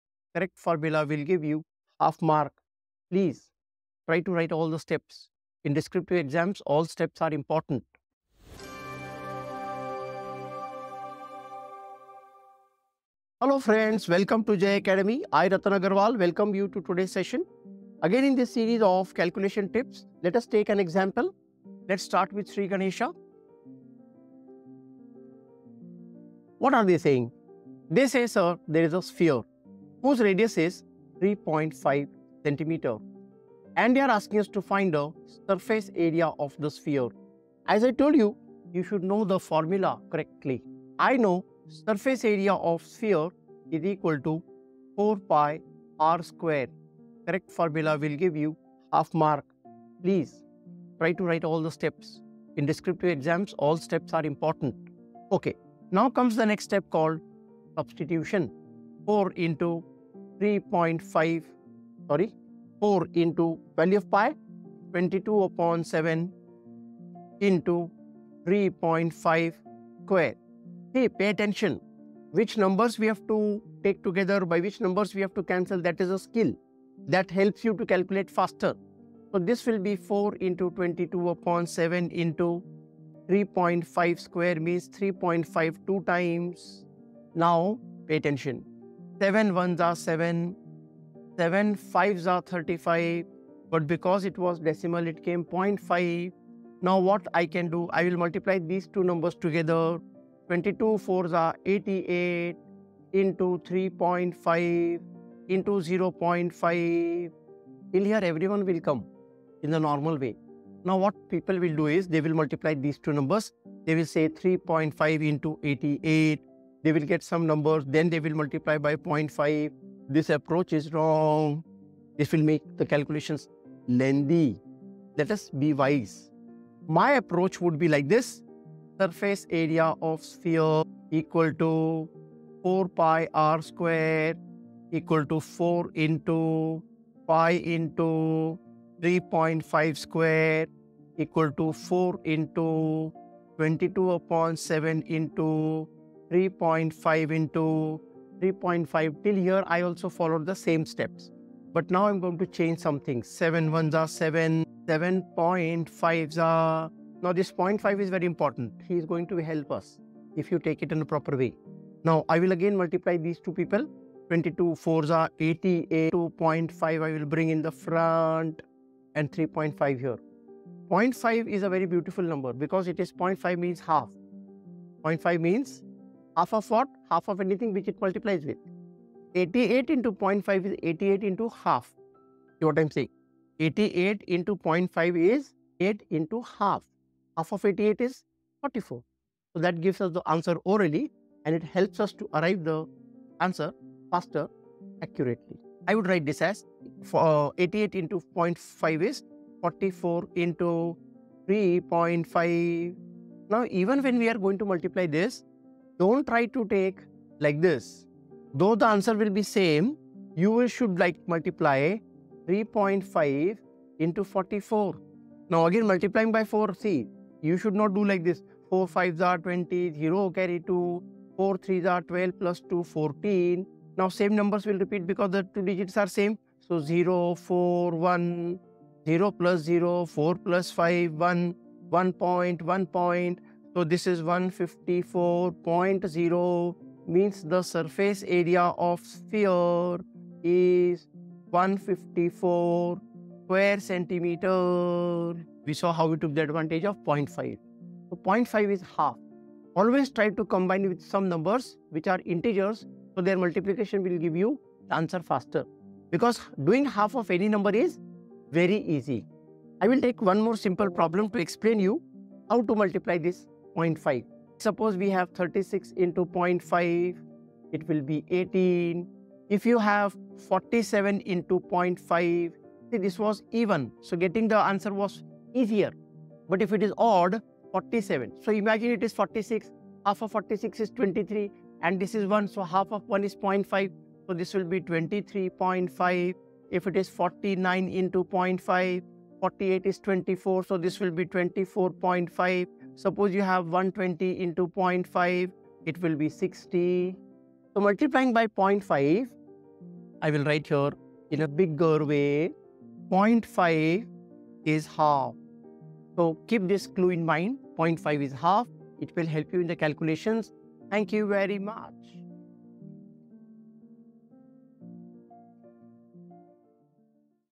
Mastering Sphere Calculations: Unlock the Power of 0.5 for Quick Math
Welcome to another exciting episode of J Academy where we delve into some effective calculation tips. Today, I, Ratan Agarwal, break down the seemingly complex task of finding the surface area of a sphere. The magic trick? Using 0.5 to simplify your calculations and get answers faster! Don't miss out on this math hack that can save you time in exams.